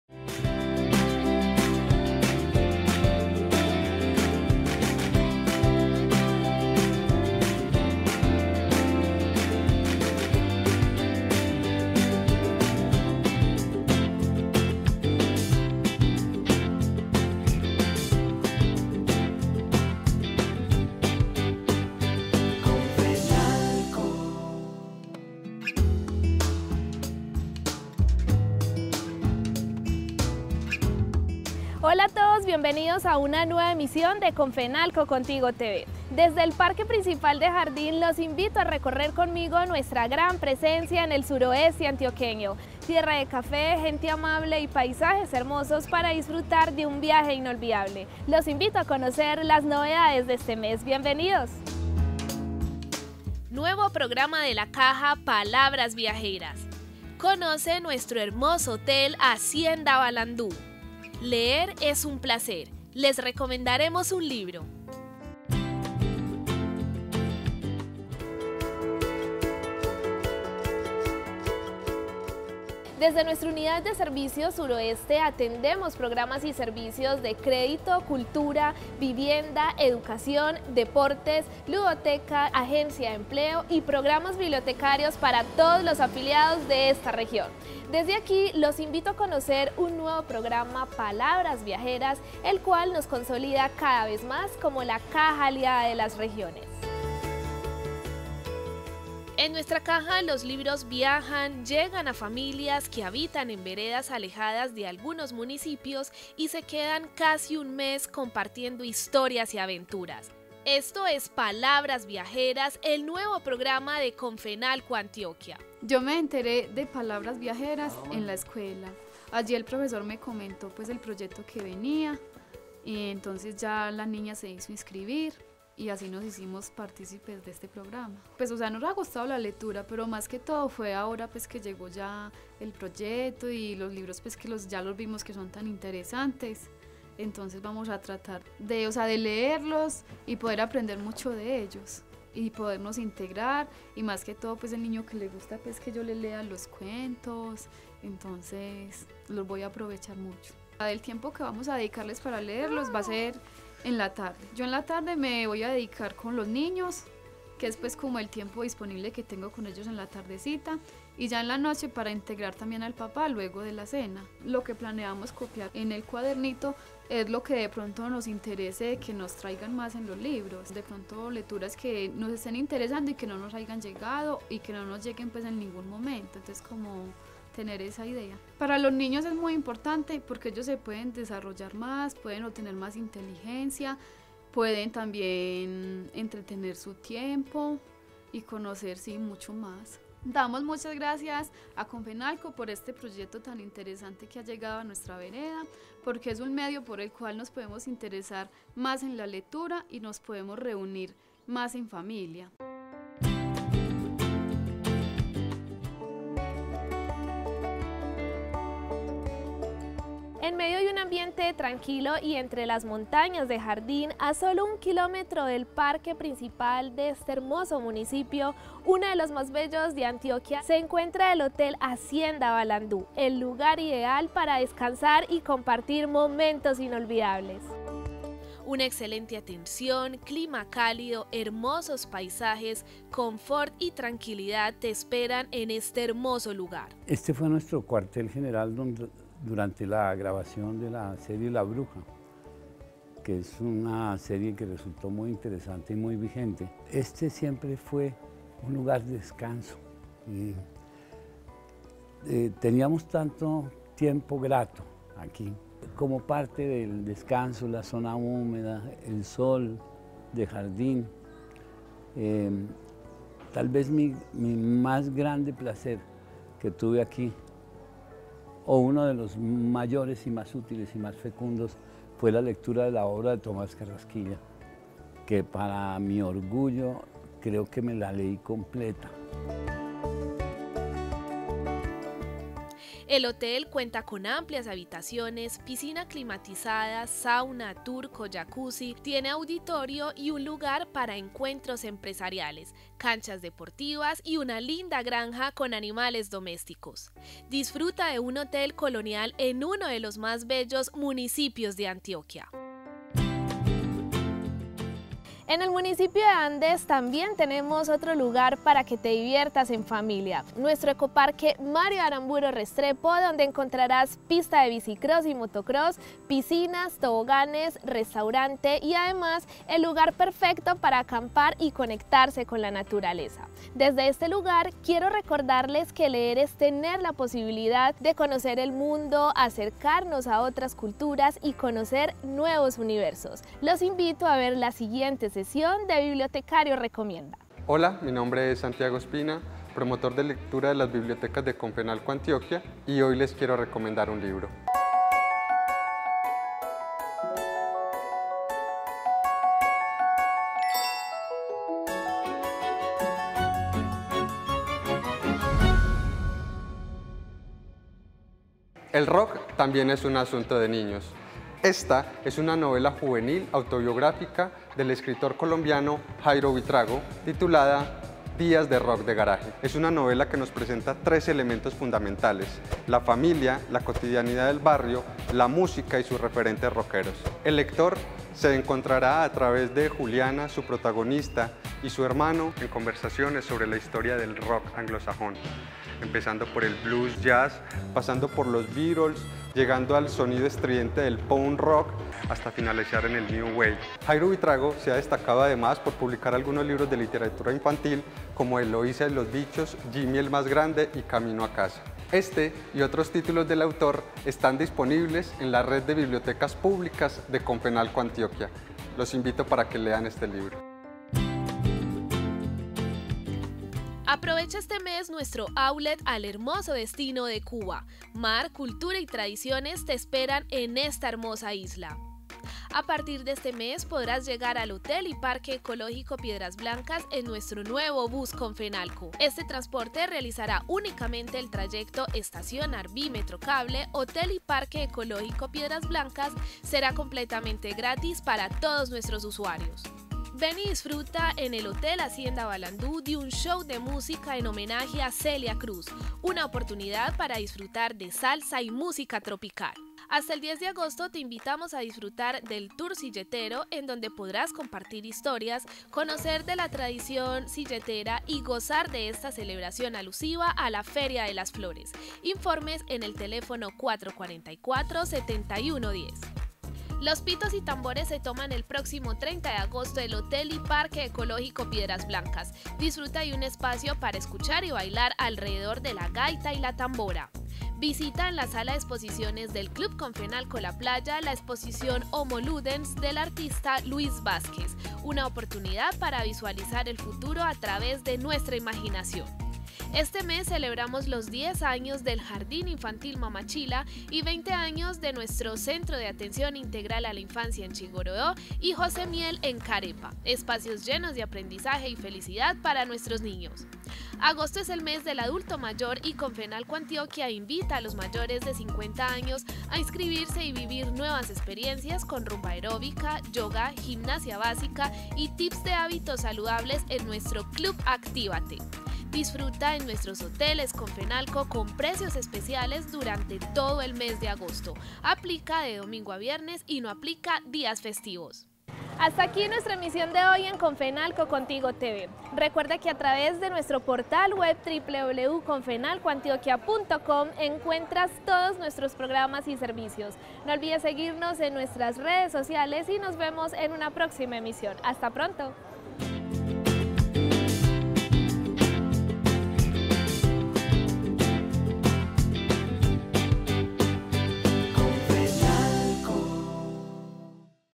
Suroeste antioqueño, recorre con nosotros nuestra presencia en esta región [Comfenalco Antioquia]
Conoce en esta emisión nuestra gran presencia en el suroeste antioqueño. Tierra de café, gente amable y paisajes hermosos para disfrutar de un viaje inolvidable. Entérate de las novedades de este mes.